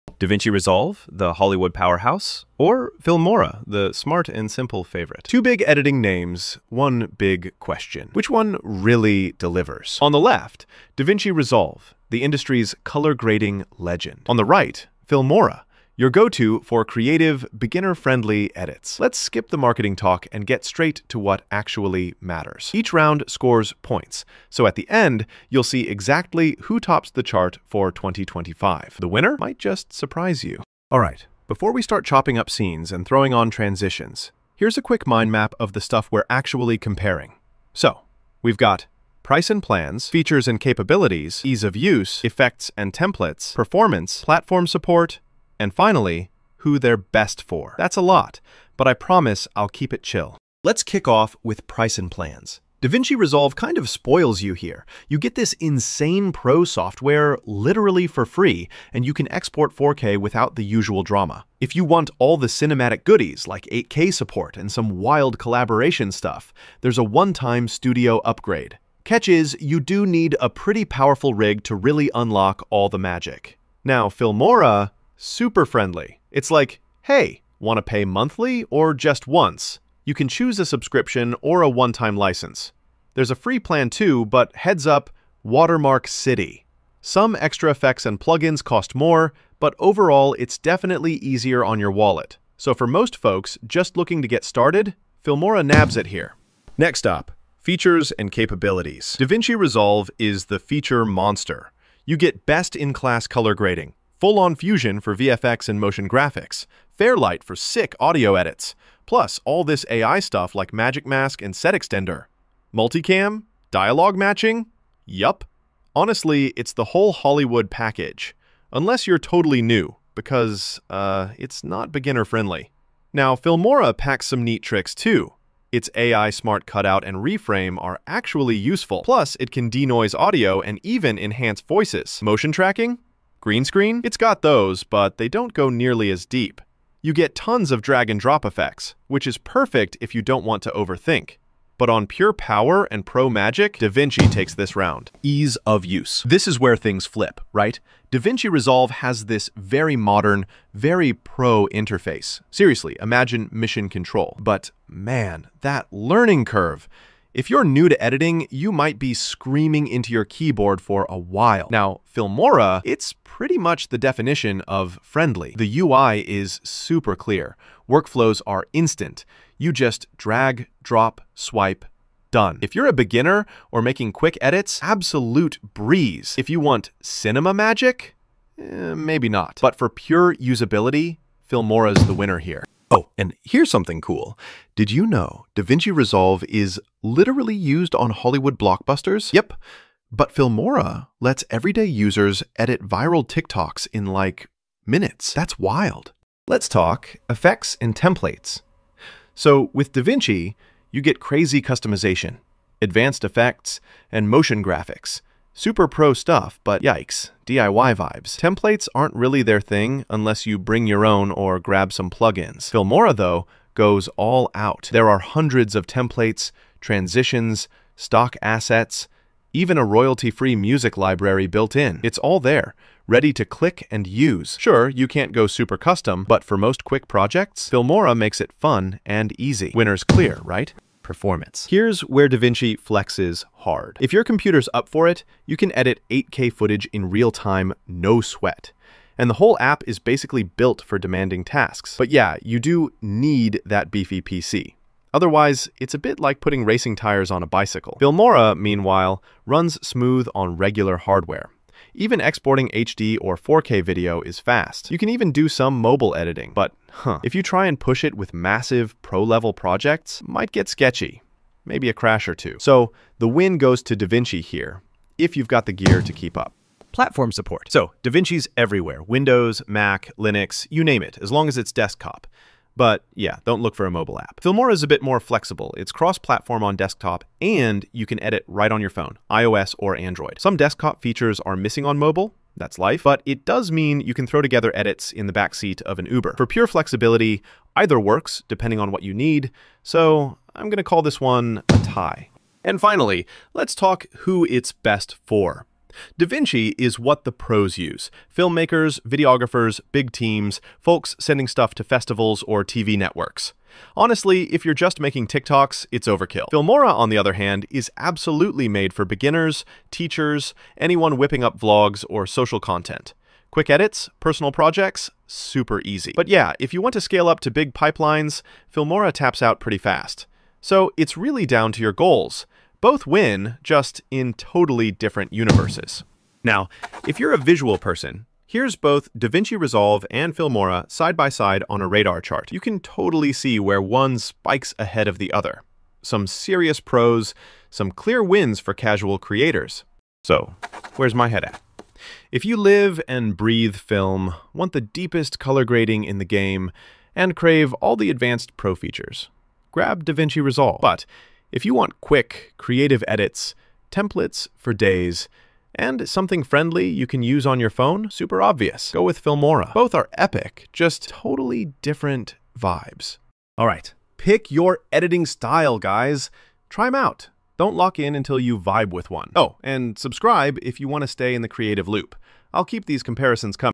DaVinci Resolve vs Filmora Comparison | Best Video Editing Software 2025
Looking for the best video editing software in 2025? In this tutorial, we compare DaVinci Resolve and Filmora across features, usability, pricing, and platform support to help you find the perfect editor for your needs.

Timestamps:
00:00 Introduction
01:12 User Interface Comparison
04:30 Features & Effects Breakdown
08:15 Performance & Export Quality
11:00 Pricing & Licensing
13:20 Supported Platforms (Windows, Mac, Linux)
15:00 Who Should Use DaVinci Resolve?
16:40 Who Should Use Filmora?
18:00 Summary & Recommendation

We cover essential aspects including editing capabilities, color grading, audio tools, built-in effects, transitions, and ease of use. If you’re deciding between DaVinci Resolve and Filmora for YouTube, professional filmmaking, or simple home videos, this video will guide your choice!